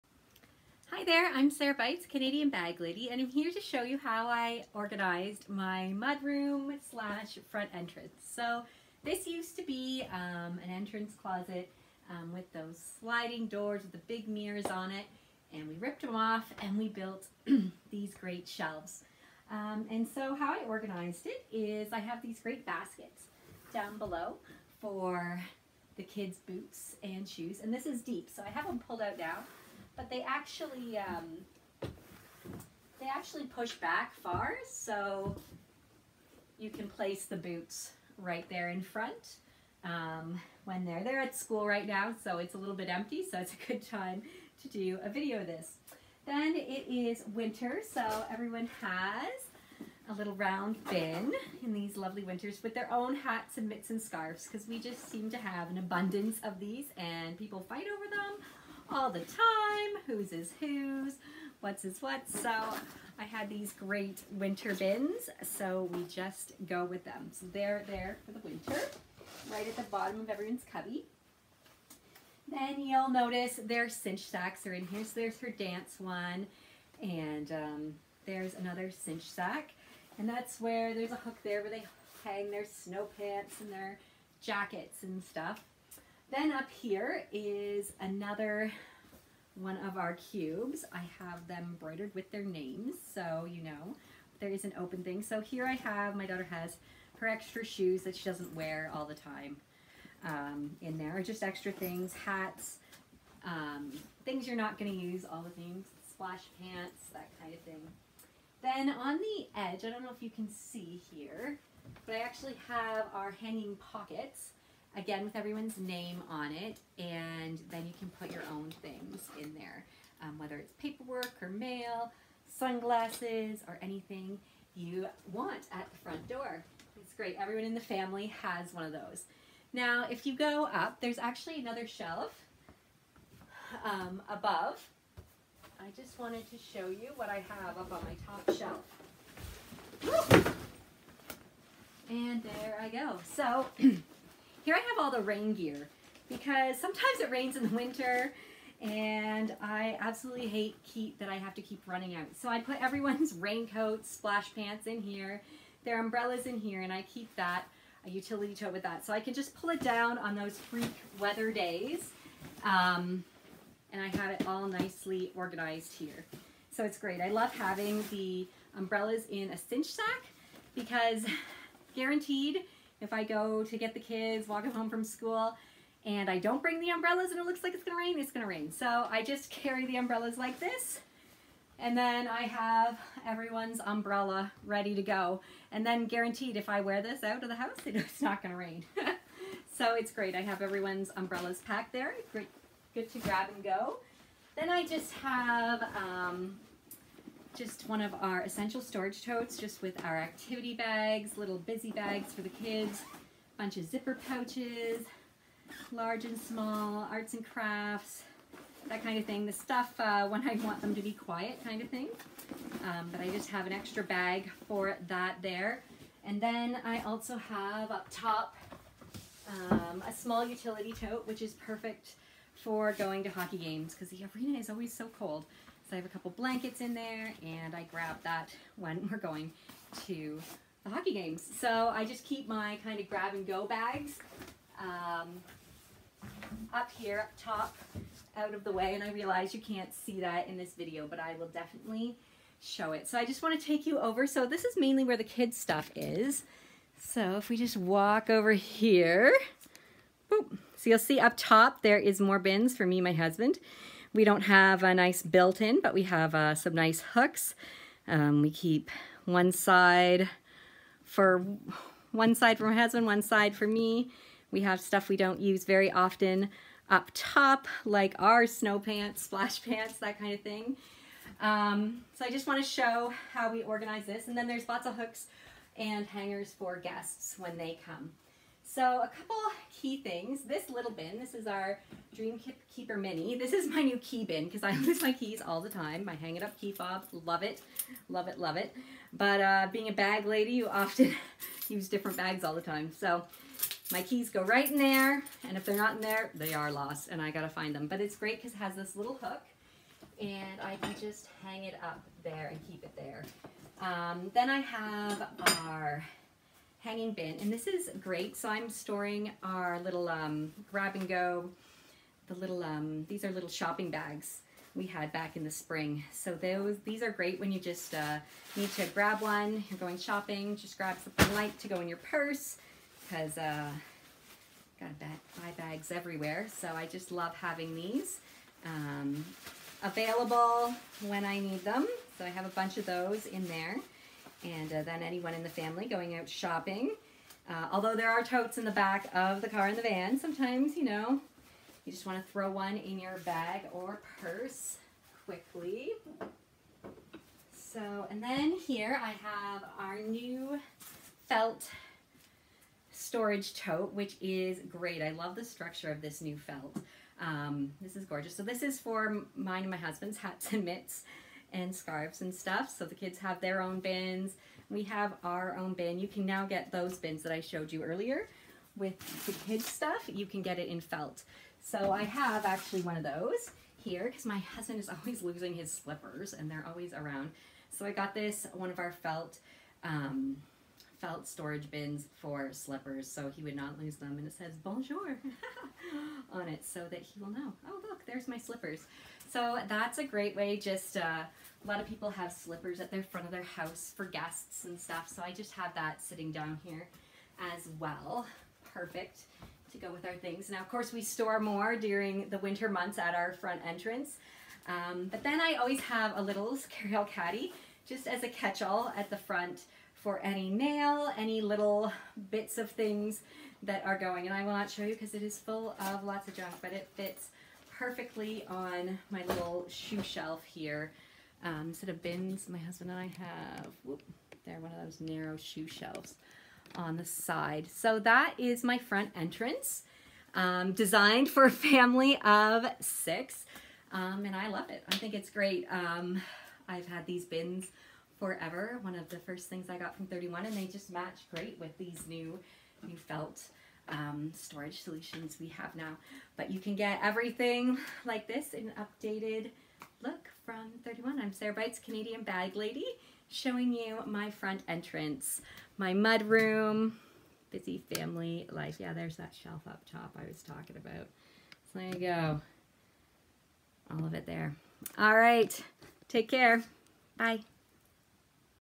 Operation Organization - entrance/mud room edition
I love organizing with thirty-one gifts! See how I keep my front entrance functional and fabulous. My front entrance features your way cubes, your way rectangles, felt Storage bin, felt hanging bin, DreamKeeper mini, felt Storage Tote, littles carry-all caddy, cinch sacs, and Utility totes too!